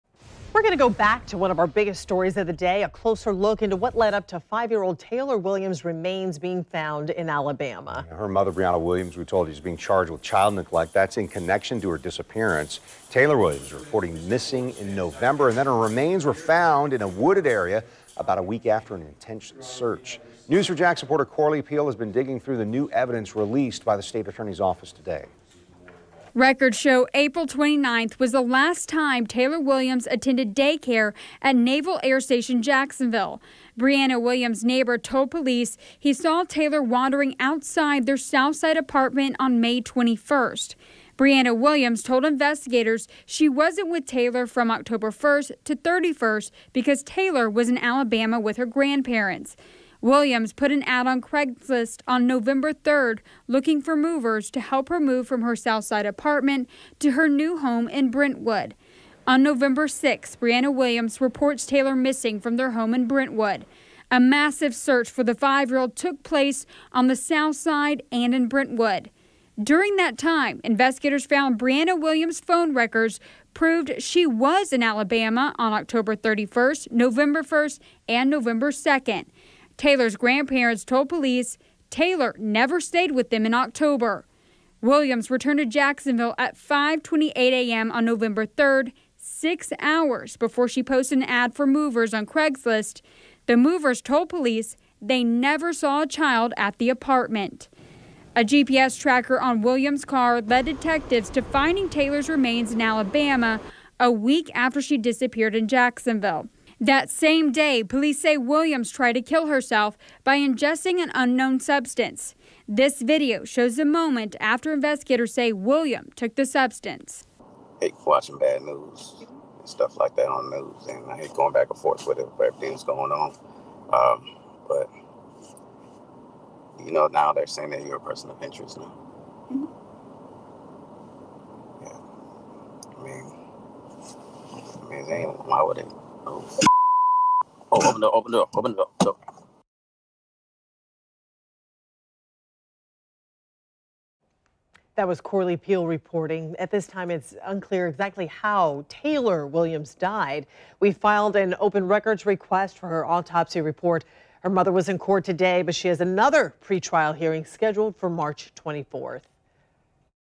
Scent of decay led detectives to ugly truth, remains of Taylor Williams
Prosecutors release evidence in case against 5-year-old’s mother, Brianna Williams